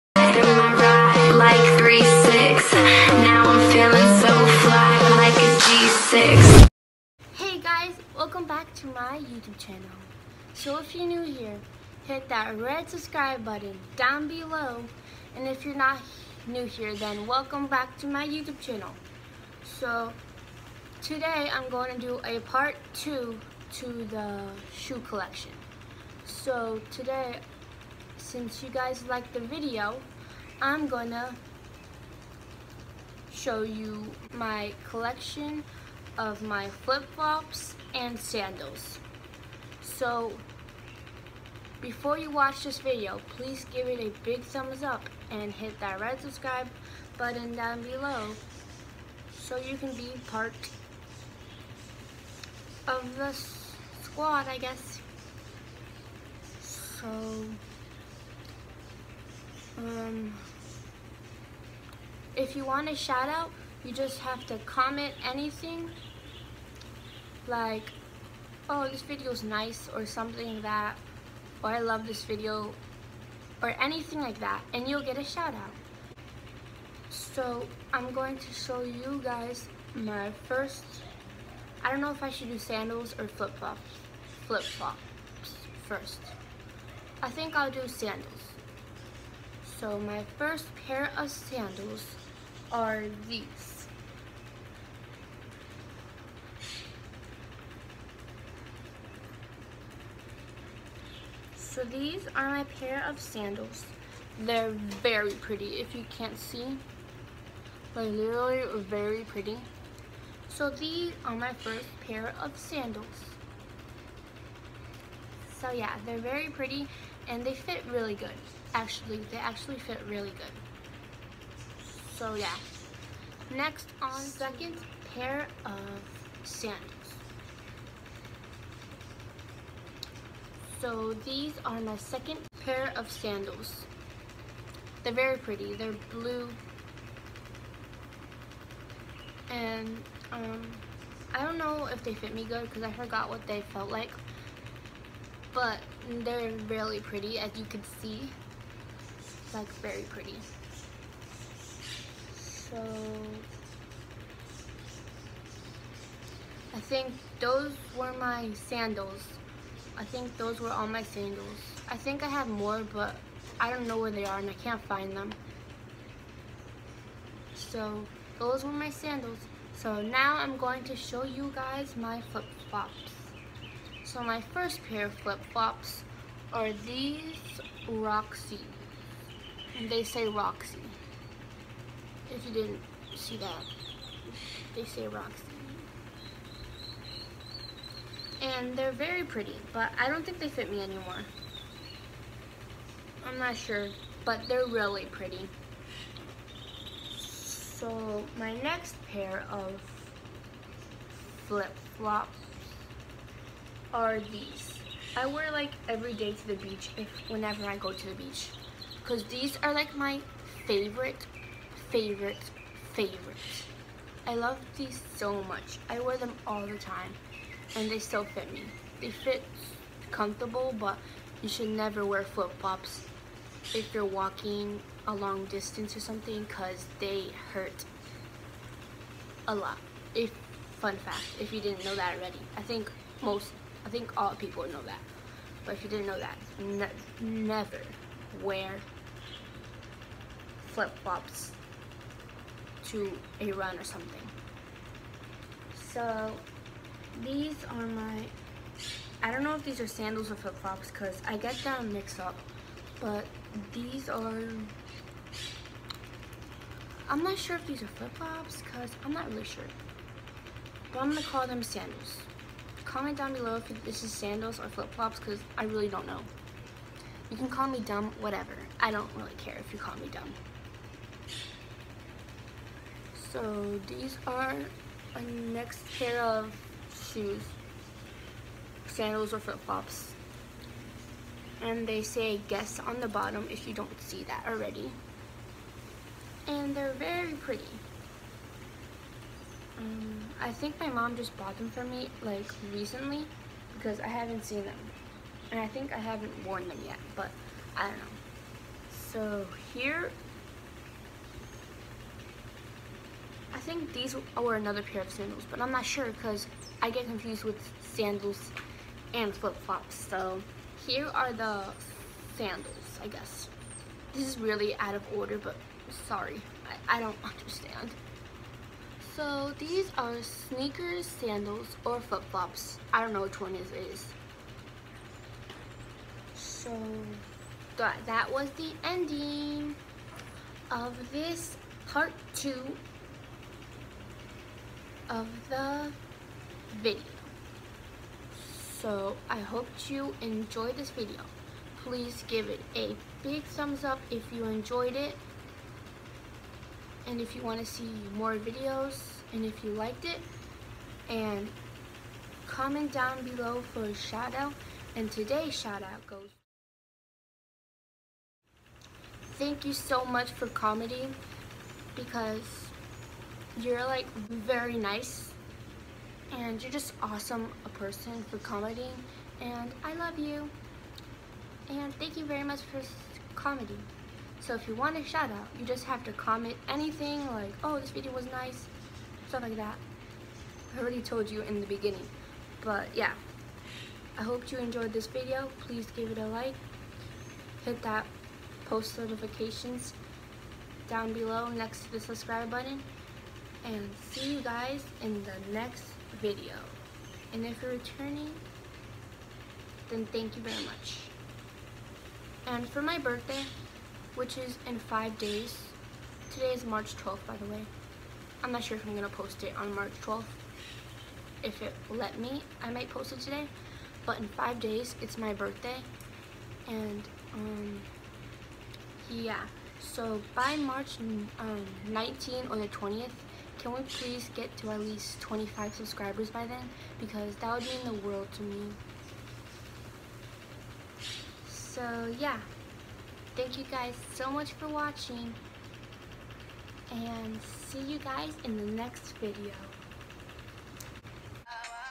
Shoe Collection Part 2
Since you guys liked the video I’m going to show you my part 2 to my shoe collection which I am showing you my sandals and flip flops.